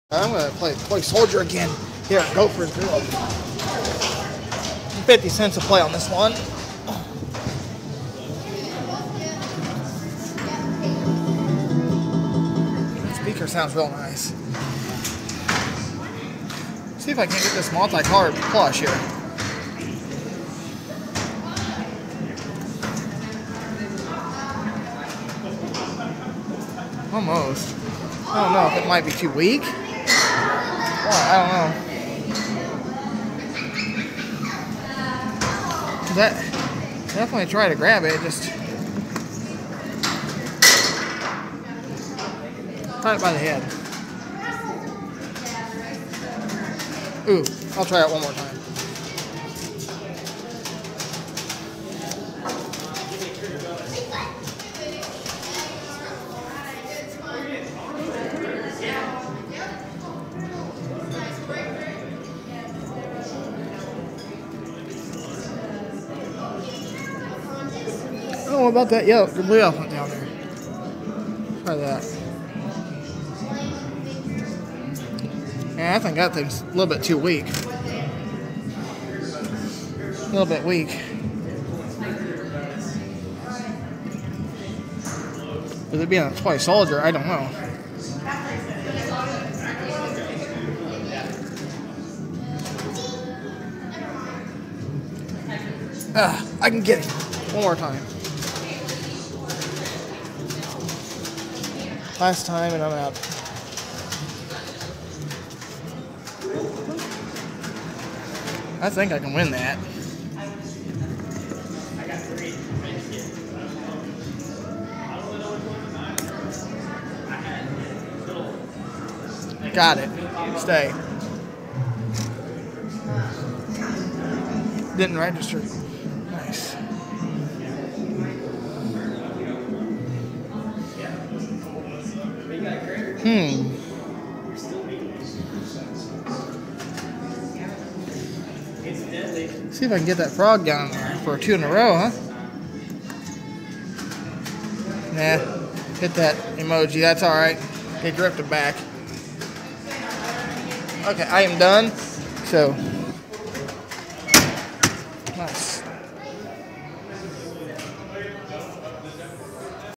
TOY SOLDIER CLAW MACHINE @ GOPHERS GRILL WEAK CLAW SPOTTED ALREADY
THIS WAS FILMED A FEW DAYS AGO AT MY LOCAL GOPHERS GRILL PLAY PUB AND GRUB HERE I AM PLAYING A LARGE TOY SOLDIER  AT 50 CENTS PER PLAY  NORMALLY A TOY SOLDIER CLAW THIS SIZE IS A 1 DOLLAR PER PLAY